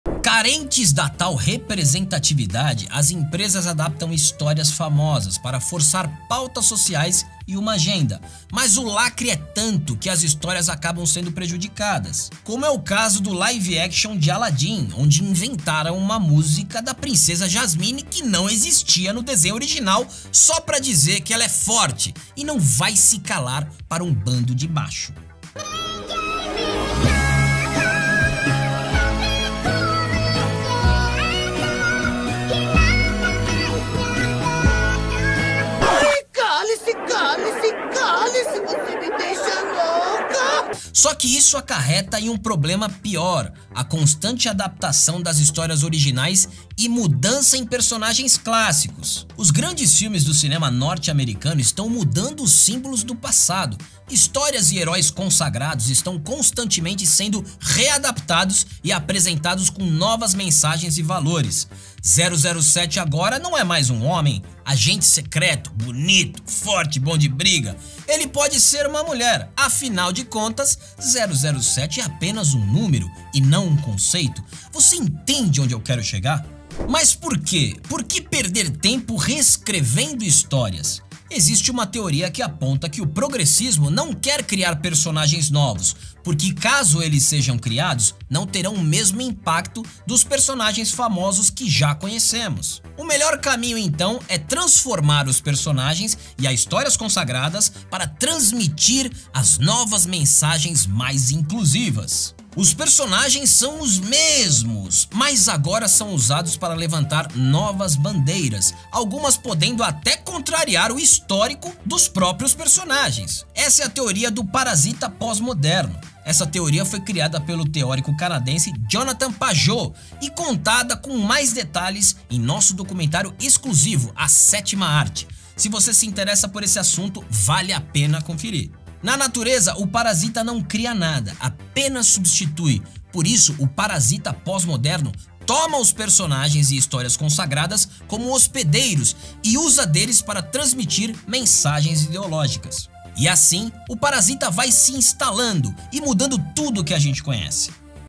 A agenda woke e a lacração em Hollywood
A agenda Woke e a lacração em Hollywood vem preocupando os fãs de obras consagradas. Com a mudança de personagens clássicos, a lacrada prejudica as obras originais?

Siga a BrasilParalelo